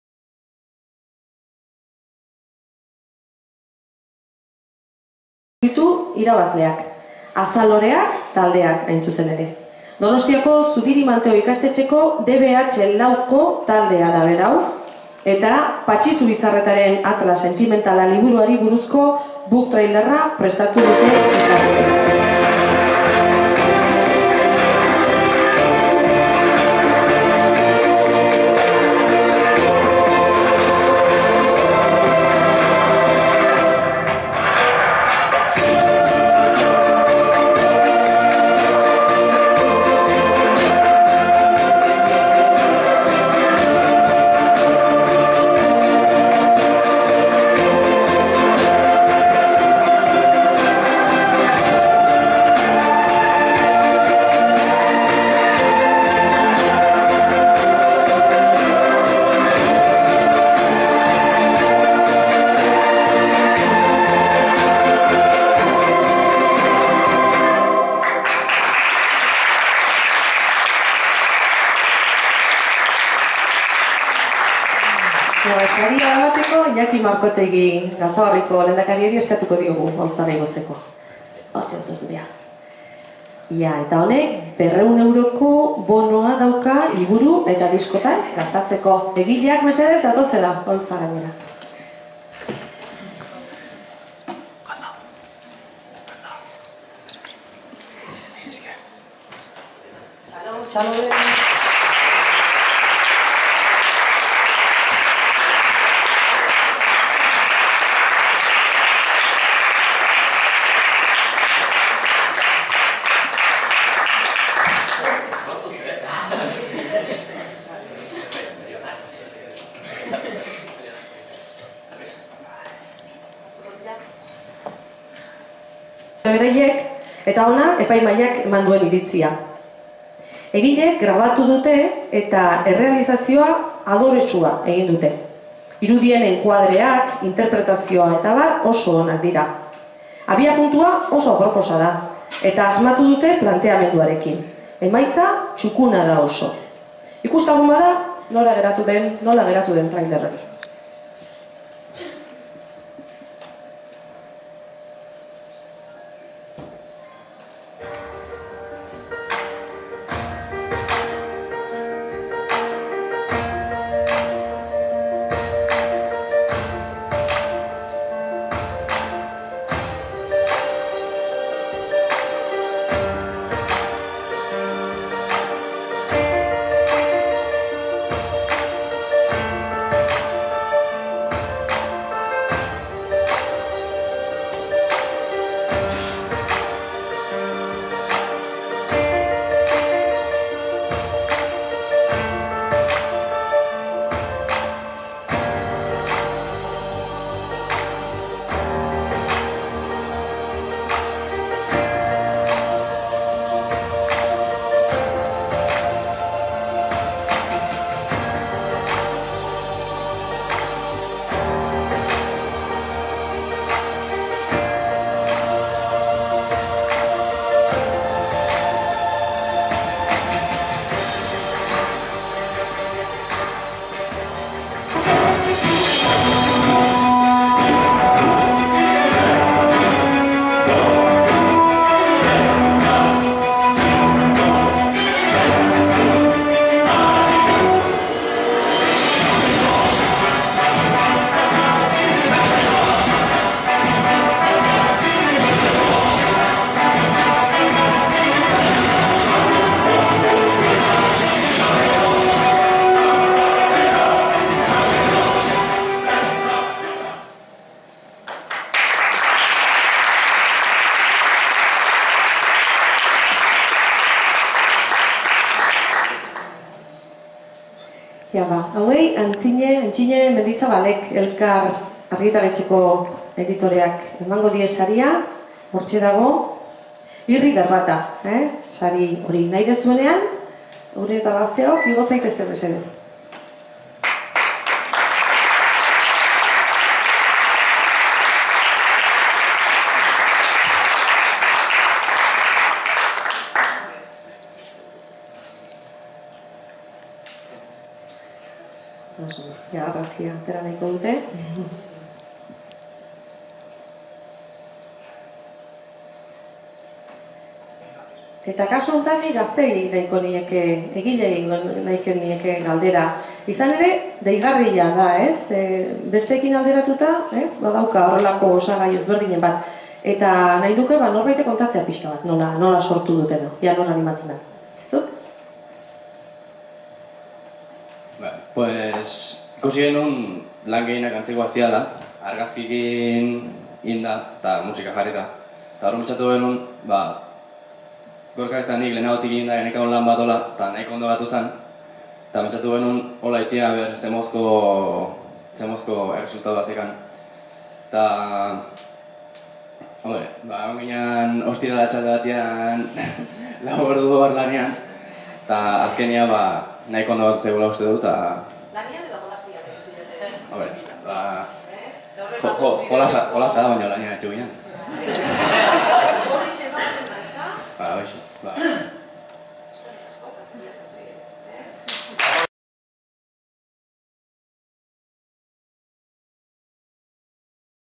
Trailerren sari-banaketa
Book Trailer Lehiaketako Publikoaren Saria Manteoko ikasleentzat

Galtzagorri elkarteak antolatutako lehiaketa da hori. Aurtengo edizioan, 4. mailako ikasleek liburu bat irakurri, trailerra sortu, eta gainera, publikoaren aitortza jaso dute. Lan txukuna Mikel Araolaza, Maider Uralde, Ane Diz eta Martin Perezek —AZALOREAK taldeak— burututakoa. Gustagarria ere Bergara, Lasarte eta Urnietako gainontzeko irabazleek erakutsitako harribitxiak. Ikusi nahi izatera, jo galtzagorri.org orrira.

El premio del público al mejor Book Trailer para Manteo

Organizado por Galtzagorri, el jueves día 15 alumnos de 4 de la ESO —Mikel, Maider, Ane y Martin— recibieron el reconocimiento del público por el book-trailer creado en torno a la novela previamente leída Atlas sentimentala del autor Patxi Zubizarreta.